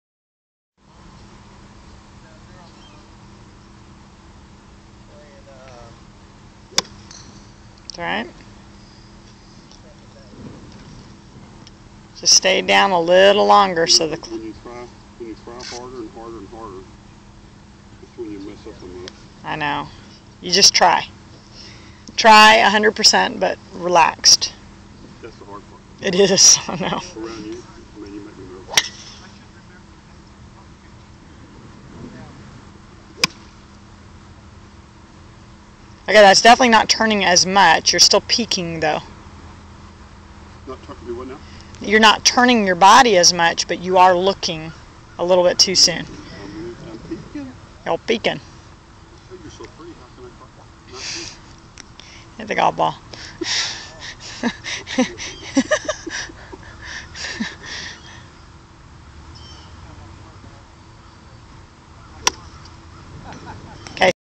Gary c/full swing with improvements (11/12/11)
arms more in front on finish